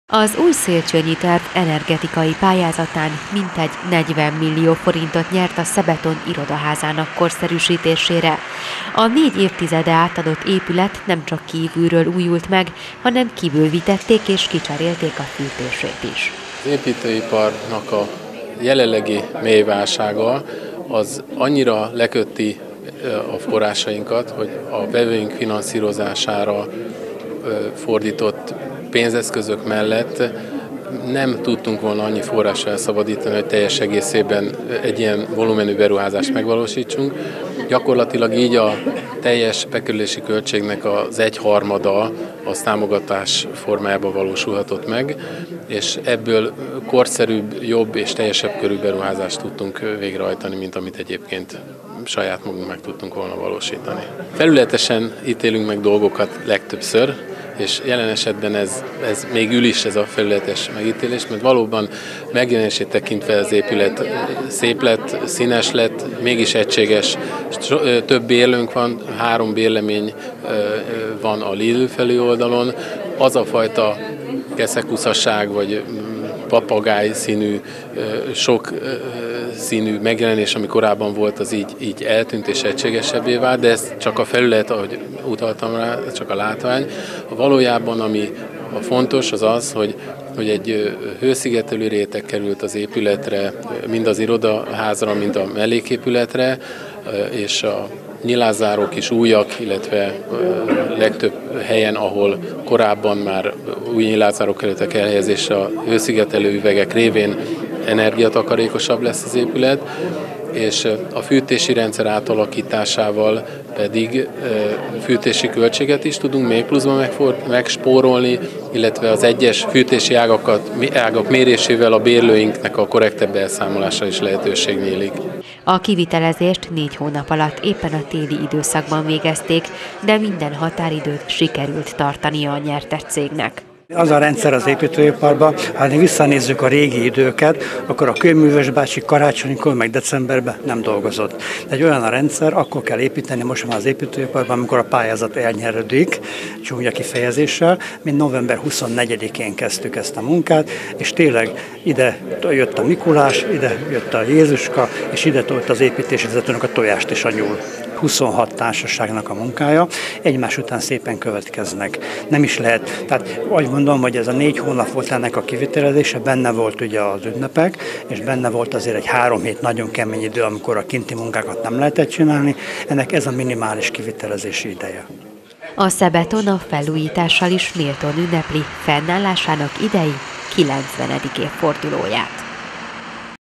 Átadták a felújított Szebeton irodaházat
120 millió forintból újították fel a Szentendrei Betonárugyár irodaépületét. A külső megújulás mellett energetikai fejlesztést is végeztek az épületen pályázati forrás igénybevételével.

TV Szentendre 2012